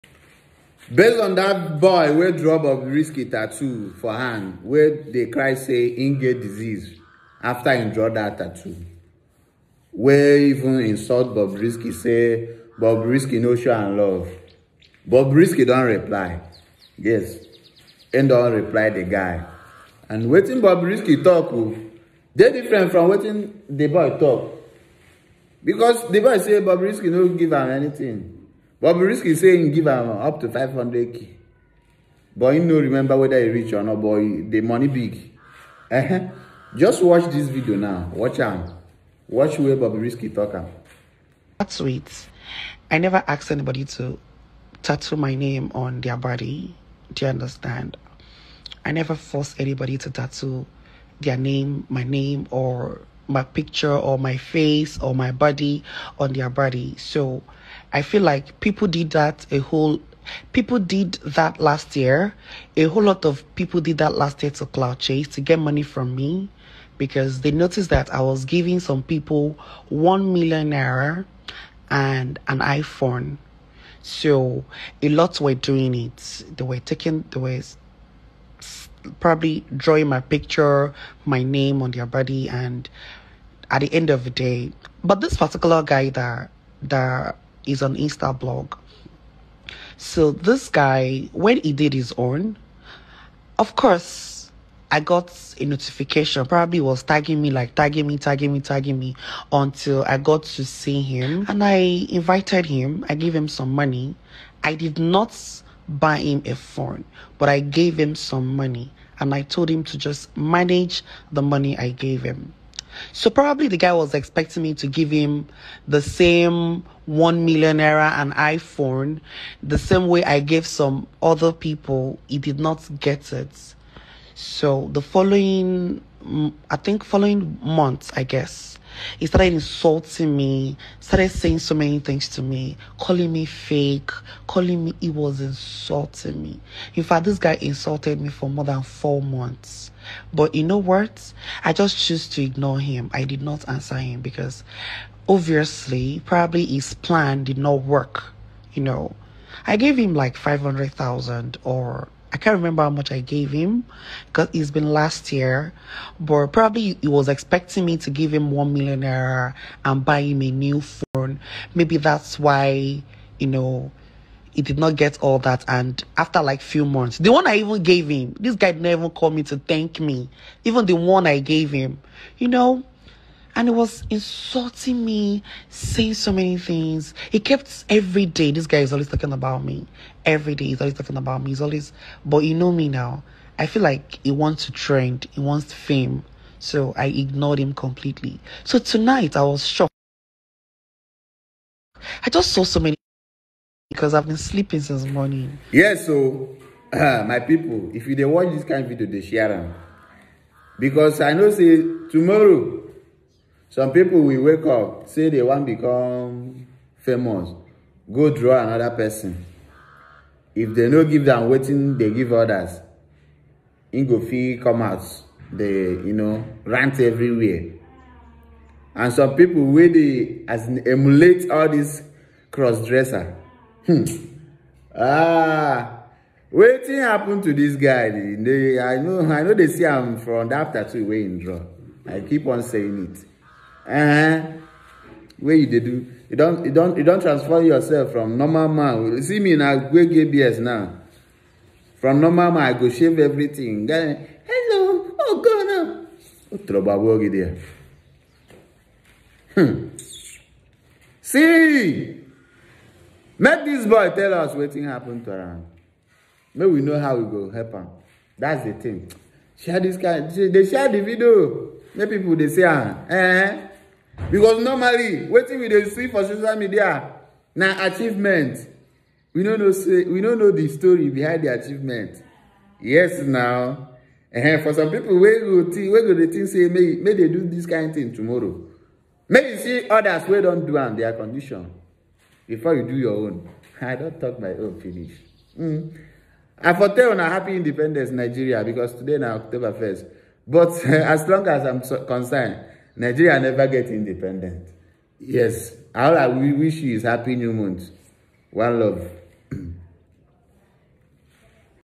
Bobrisky replies the boy that Got disease From tattooing him.
Shocking reply from Bobrisky to the boy that got disease from tattooing him on his arms